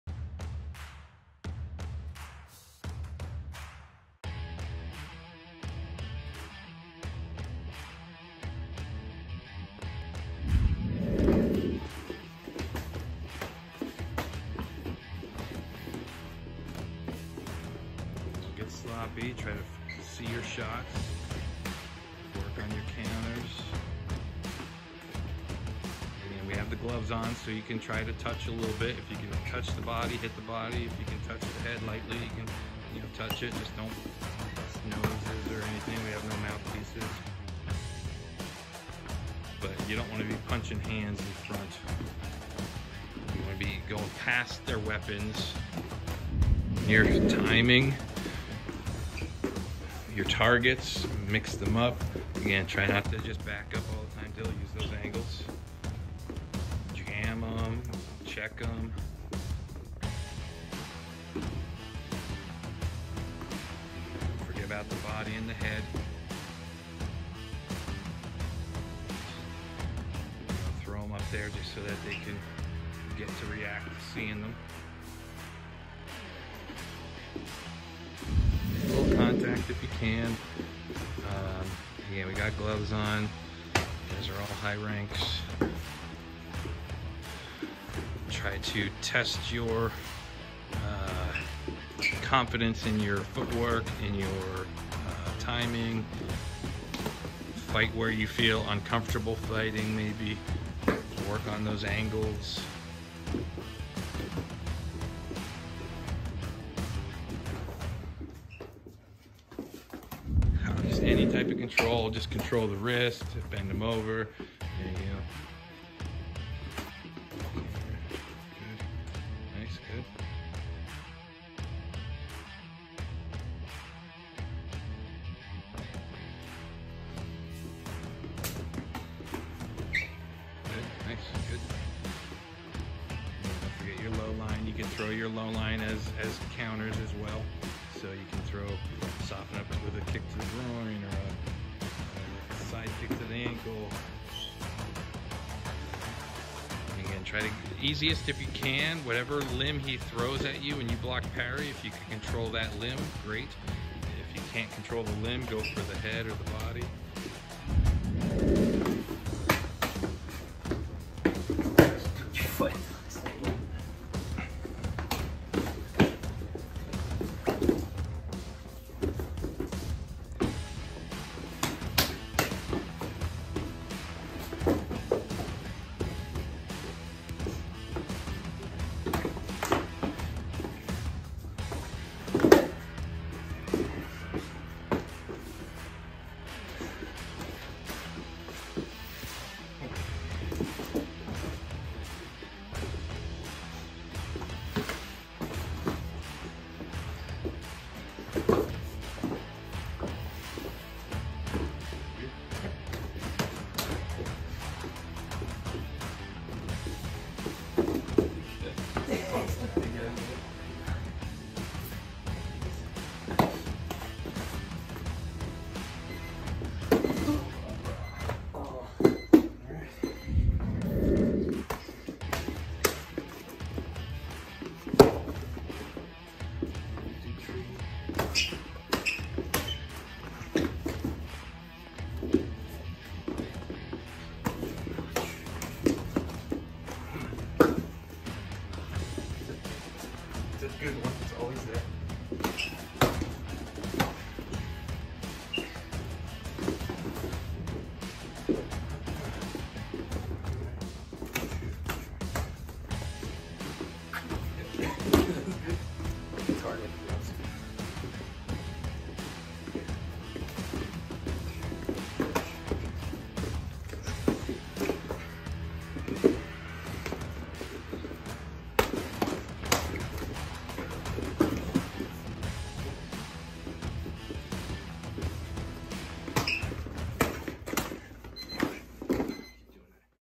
Mountaintop Martial Arts. Self Defense Drill. Karate-Do & Aiki Jutsu.
Mountaintop Martial Arts. Self Defense Drill. Karate-Do & Aiki Jutsu. (Hands & Kicks. Block/Parry & Takedowns)